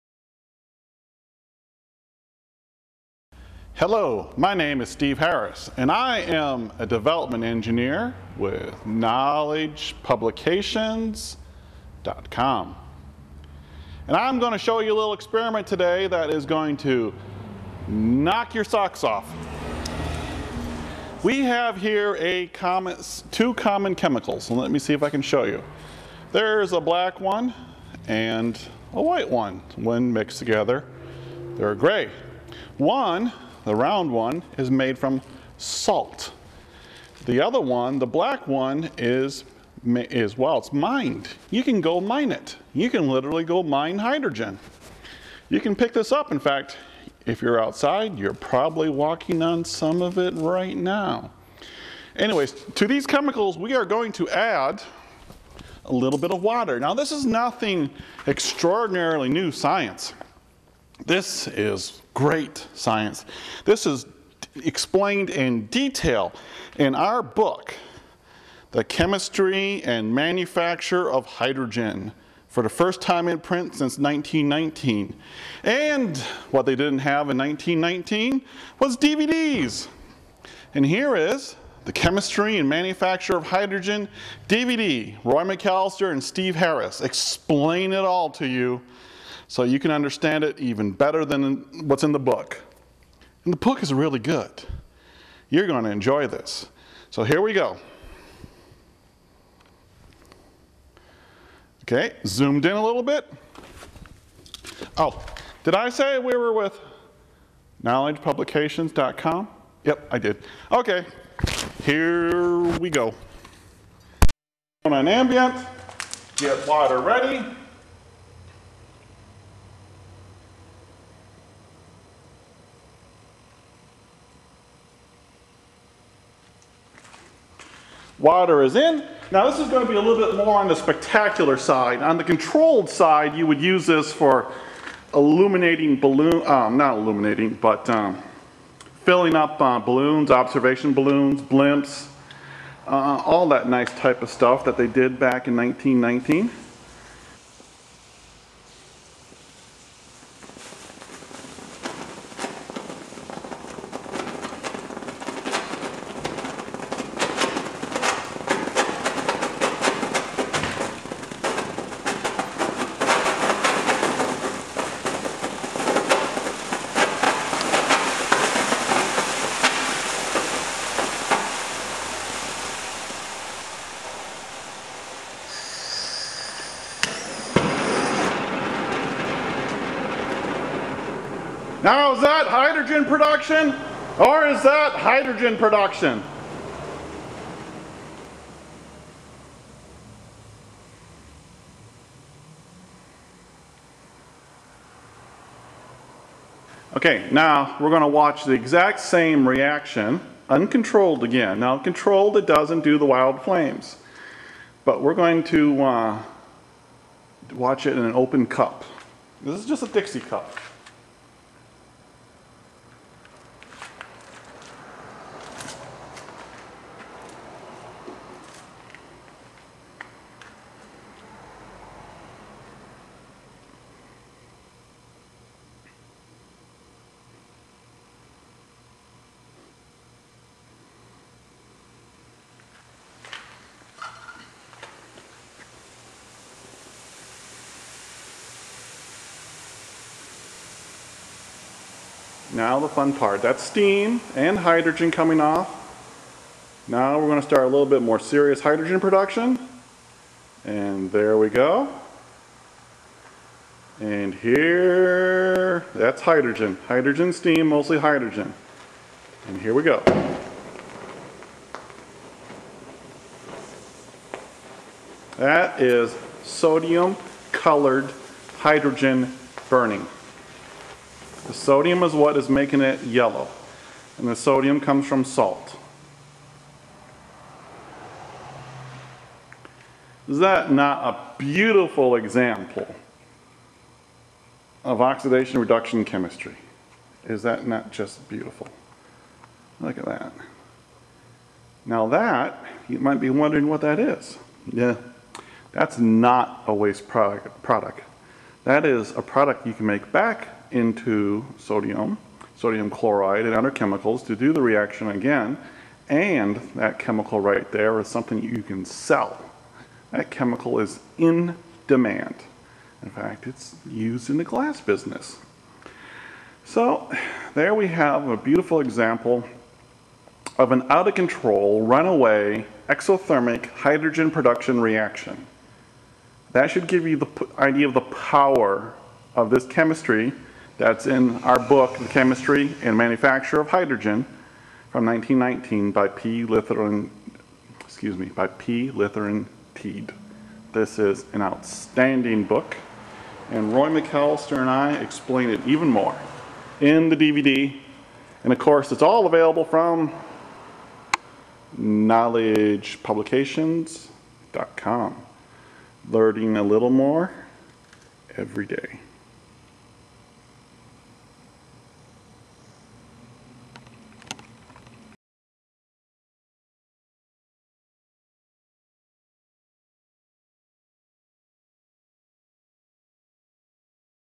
On Demand HUGE Hydrogen Production, How to Make Hydrogen
How to Make Hydrogen, Hydrogen Production from simple and inexpensive chemicals as demonstrated in our Book and DVD